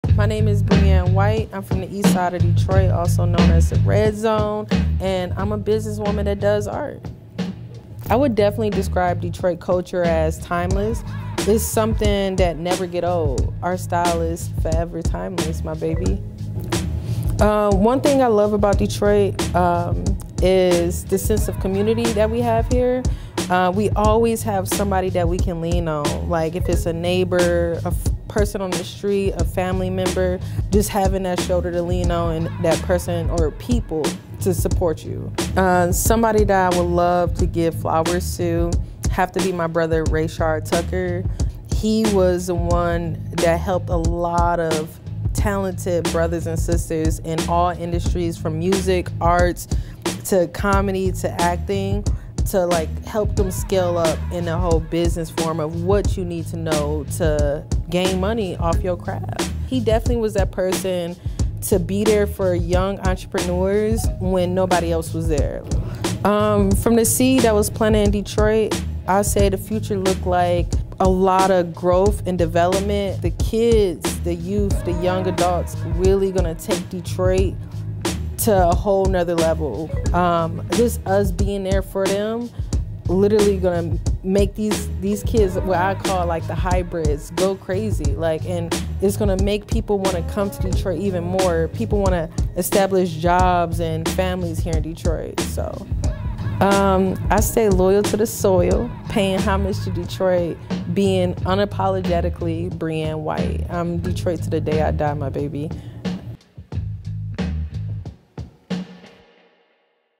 Give 'Em Flowers - Breann White
Through a lens Breann White has captured an authentic and imaginative art worldwide, while simultaneously creating and cultivating striking images that draw you into the true Detroit culture.